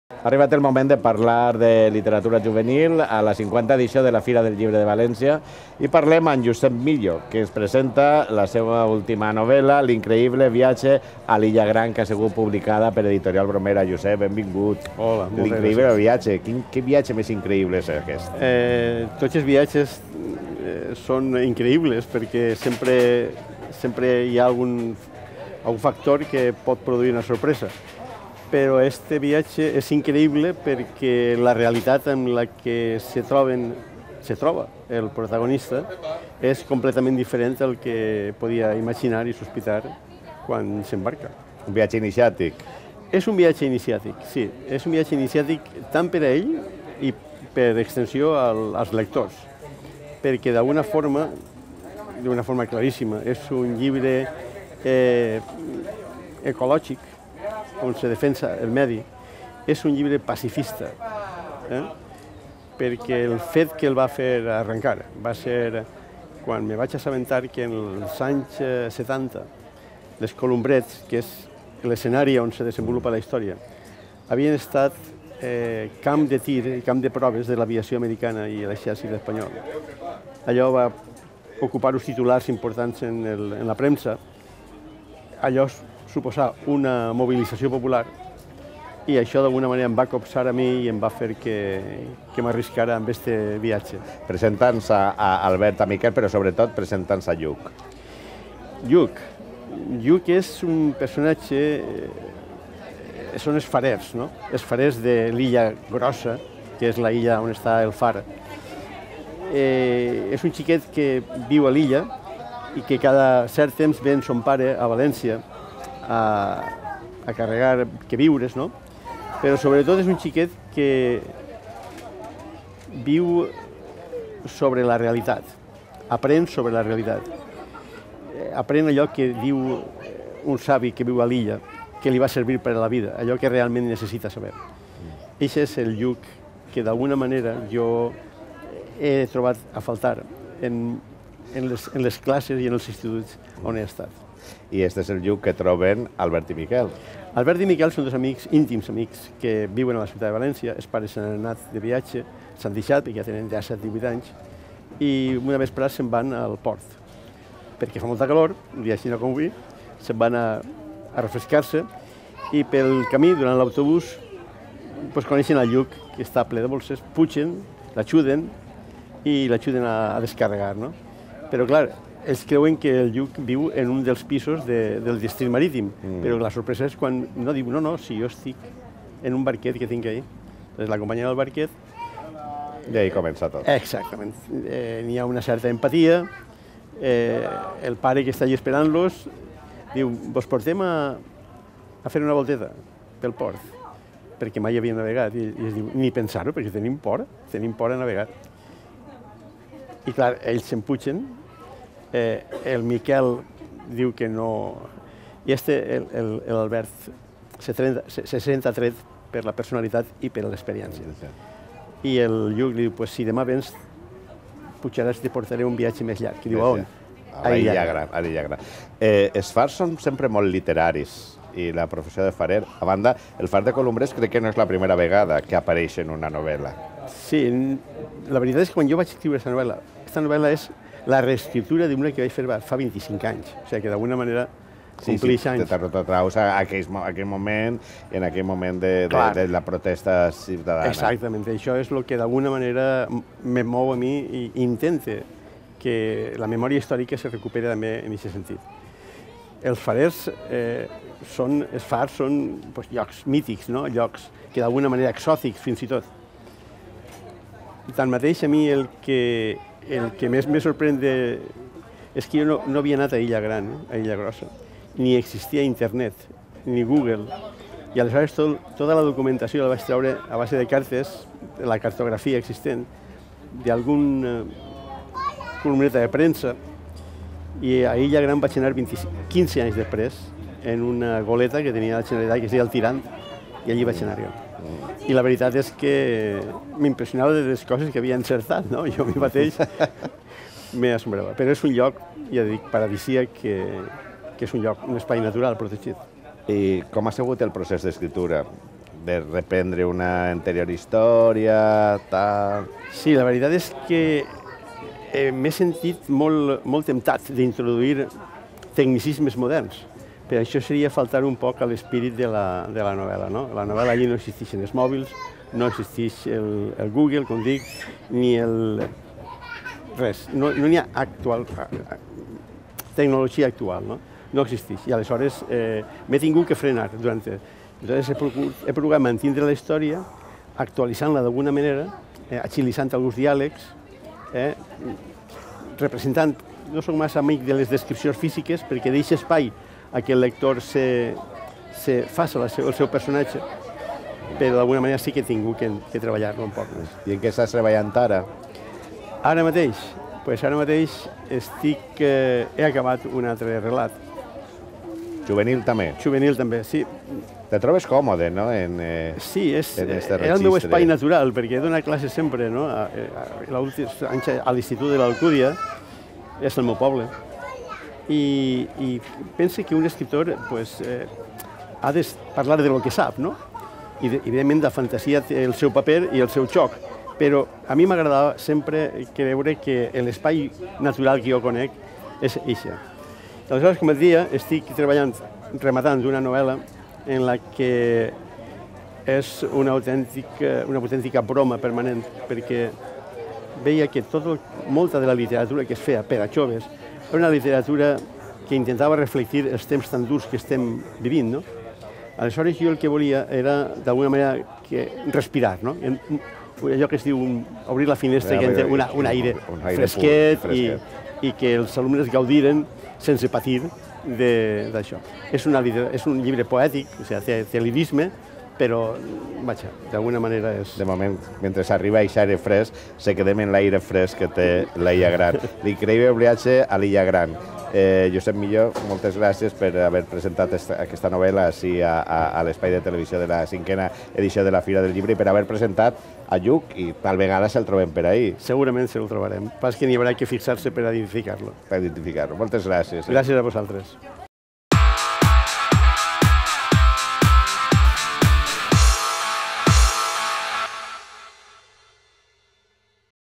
Josep Millo - L'increïble viatge a l'illa Gran
Aquesta novel·la narra com Albert i Miquel aconsegueixen escapar-se de fer un creuer amb els pares i es queden sols a casa. Mentre es preparen per a viure l’aventura de l’estiu anant-se’n d’acampada amb els amics, coneixeran Lluc, un jove sorprenent que descobreix el món per si mateix. Procedent de l’Illa Gran, un lloc del qual no han sentit parlar mai, Lluc els farà prendre una decisió que podria canviar el rumb de les seues vides.